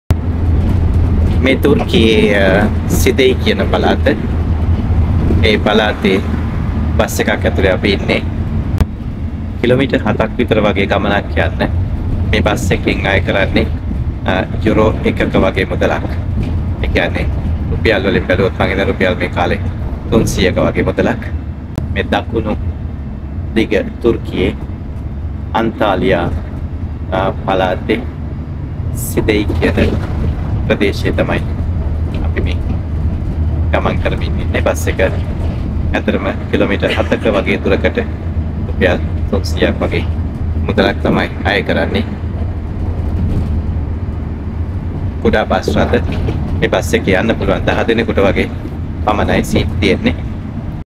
තුර්කියේ සිදෙයි නගරයේ බස් සවාරියක් | Bus journey in Side, Turkey
A bus journey in Side, Antalya, Turkey